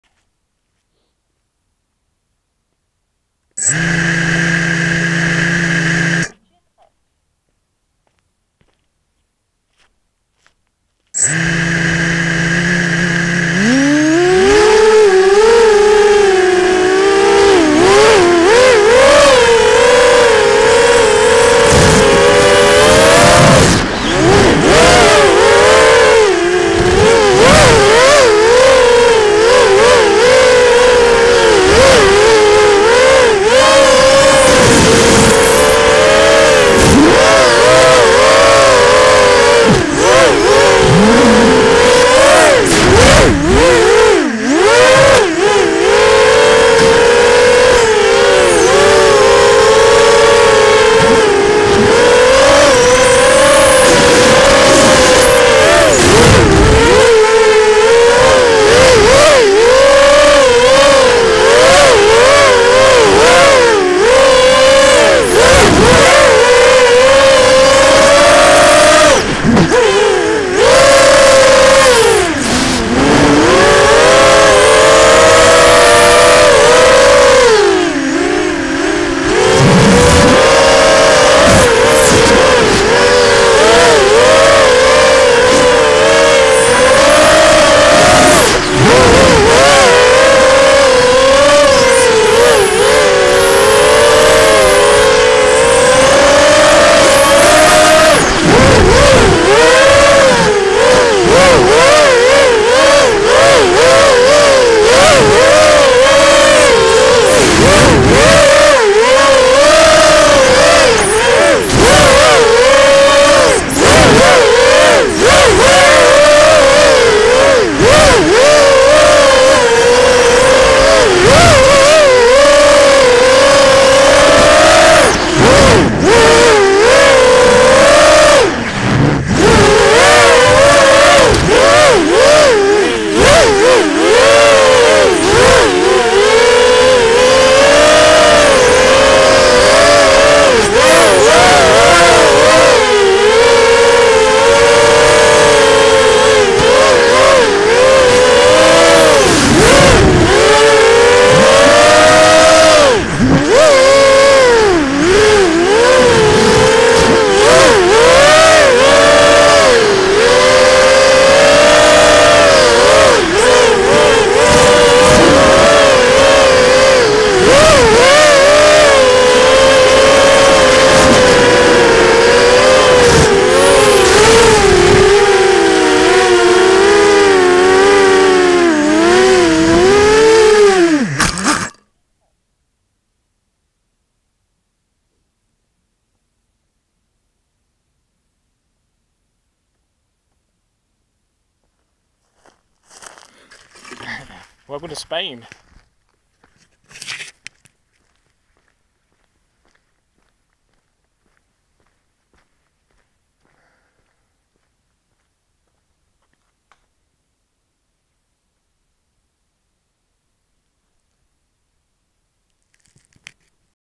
SP Racing F3 EVO on unused roads in Spain.
Found a new flying spot today!  In Spain loads of new roads were built ready to build new houses on them, then the 2008 banking crisis happened and the homes were not built, leaving loads of unused roads on hills that go no-where and have no traffic on them.  Perfect for a bit of quad flying!

Frame: Mantis M1
FC: SPRacingF3EVO
OSD: SPRacingF3OSD
Motors: Cobra 2204/2300kv
ESCs: LittleBee 20A
Battery: 1500mAh DroneLabs
VTX & Camera: Eachine